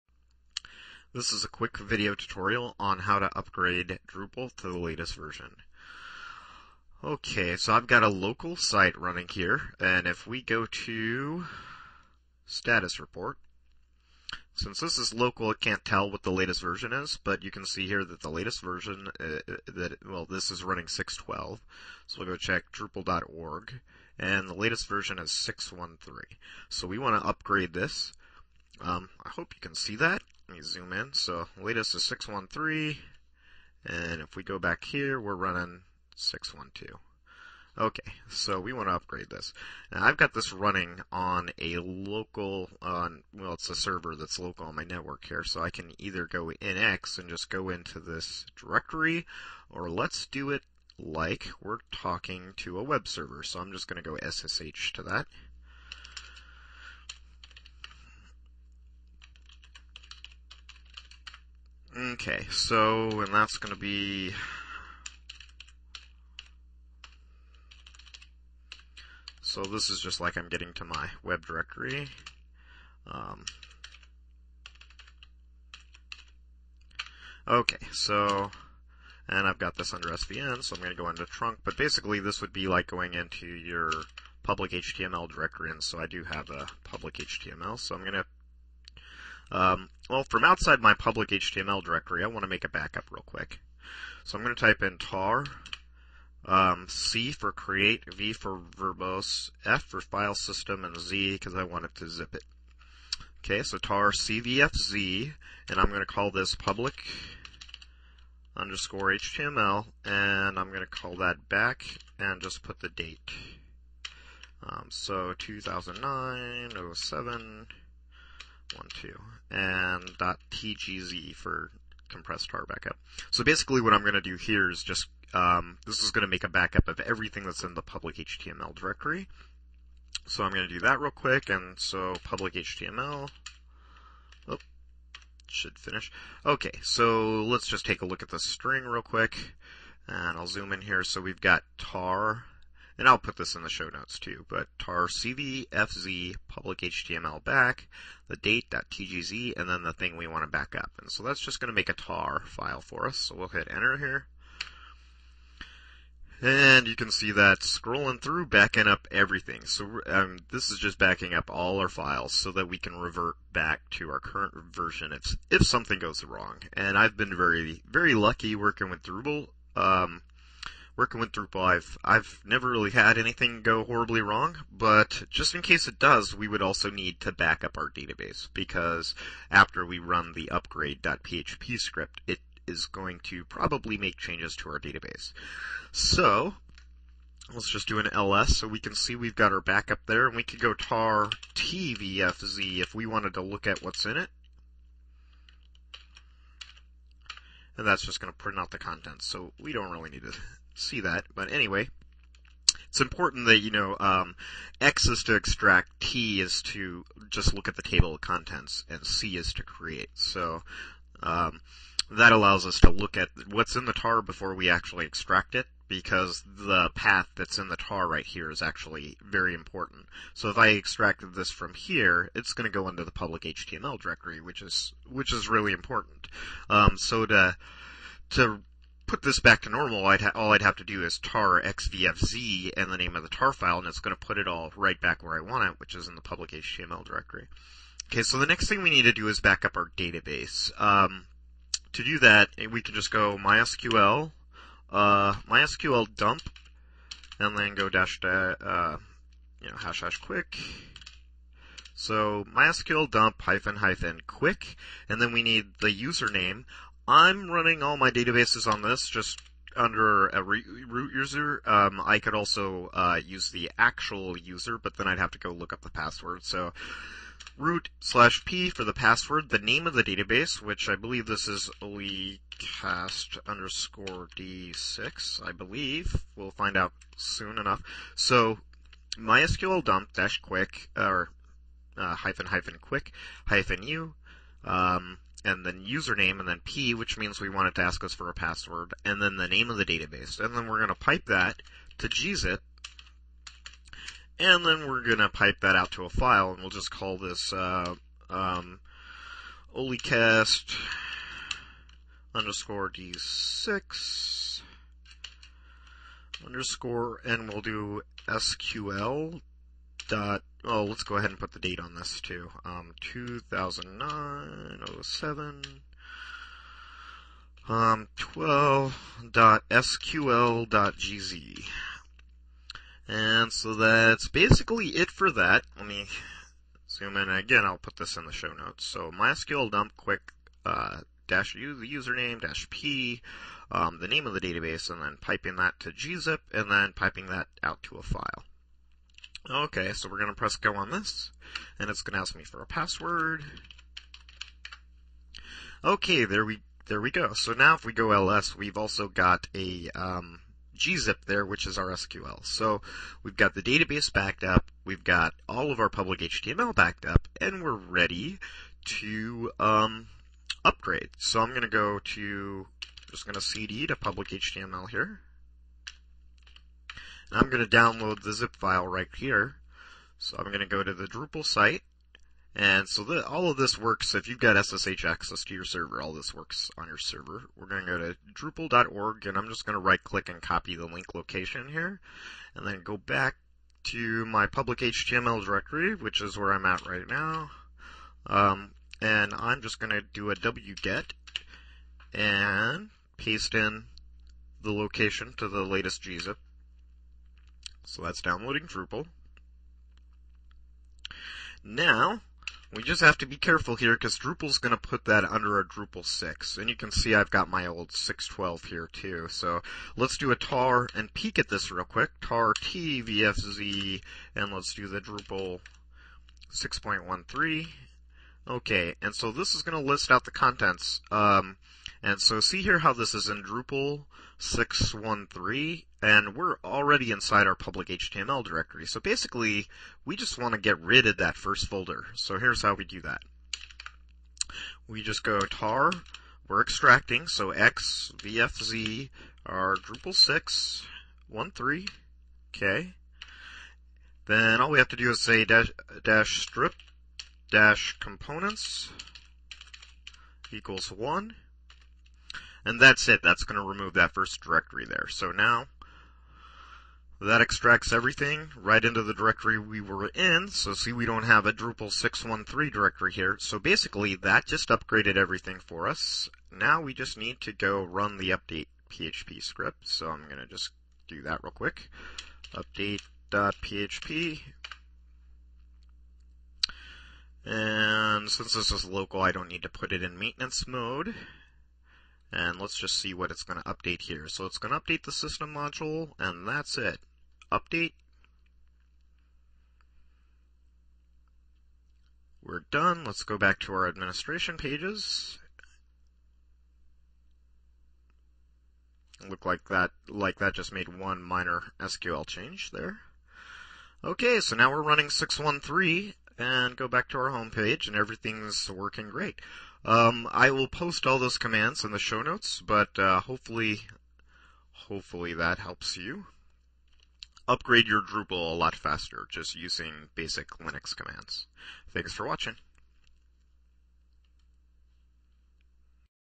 drupal update howto
How to backup and upgrade Drupal using ssh and simple shell commands

Commands used in this video:
# backup the public_html dir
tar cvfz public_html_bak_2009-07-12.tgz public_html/

# backup the database (replace username and dbname with appropriate values)
mysqldump --quick -u username -p dbname | gzip ⋗ db_bak_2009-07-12.sql.gz

# download the upgrade tar.gz file to public_html
cd public_html

# extract it
tar tvfz drupal-1.13.tar.gz --strip-components=1